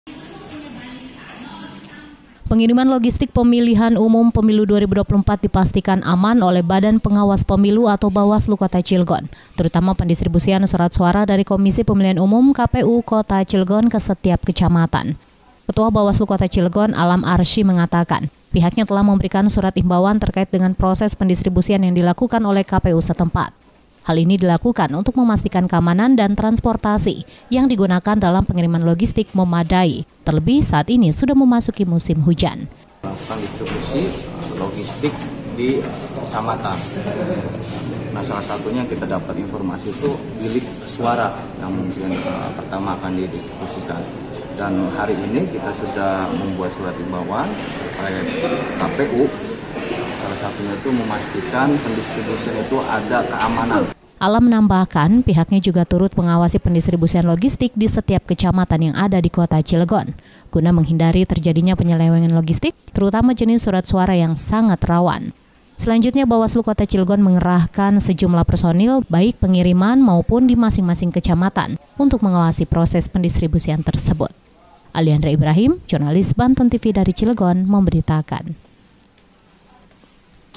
BAWASLU CILEGON PASTIKAN DISTRIBUSI LOGISTIK AMAN
BADAN PENGAWAS PEMILIHAN UMUM KOTA CILEGON PASTIKAN PENGIRIMAN LOGISTIK PEMILIHAN UMUM TAHUN 2024 AMAN/ TERUTAMA PENDISTRIBUSIAN SURAT SUARA DARI KOMISI PEMILIHAN UMUM KOTA CILEGON KE SETIAP KECAMATAN// DISTRIBUSI LOGISTIK INI JUGA AKAN DIAWASI/ GUNA MENGHINDARI TERJADINYA PENYELEWENGAN LOGISTIK//